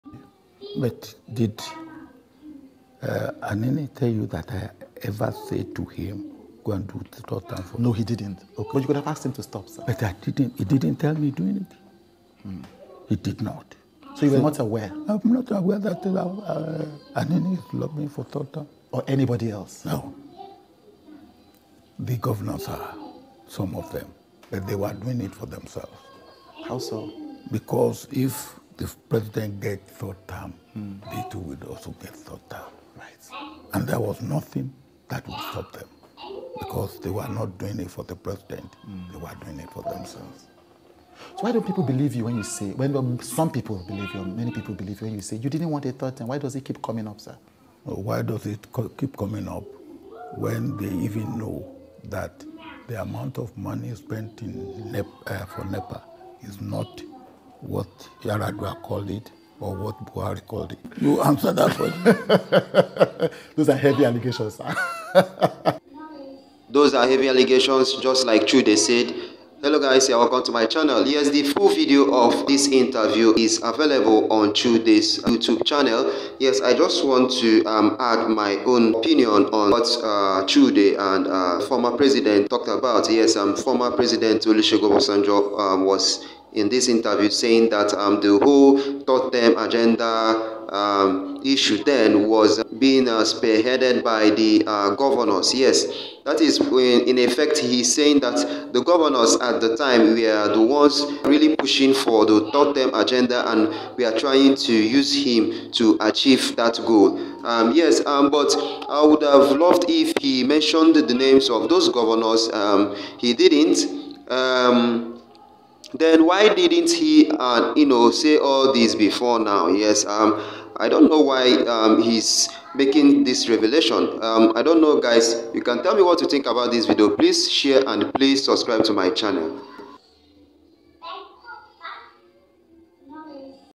Governors Were Using Me To Seek 3rd Term For Themselves - Olusegun Obasanjo
Tinubu, Atiku, Peter Obi